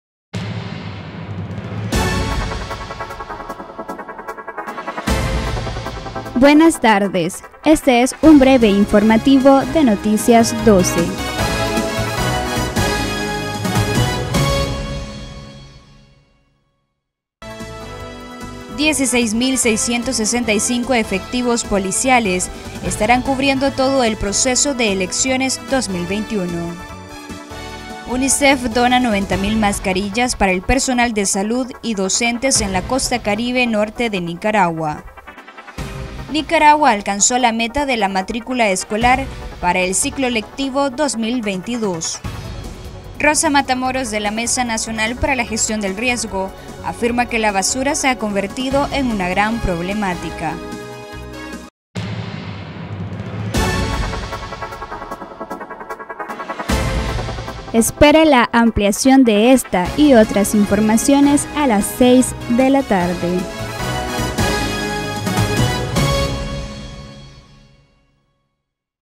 Breve Informativo
📍Más de 16 mil policías darán cobertura al proceso electoral
📍UNICEF dona 20 mil mascarillas en la Costa Caribe Sur
📍Nicaragua alcanza meta de matrícula escolar para el 2022
📍Basura continúa siendo una problemática para el país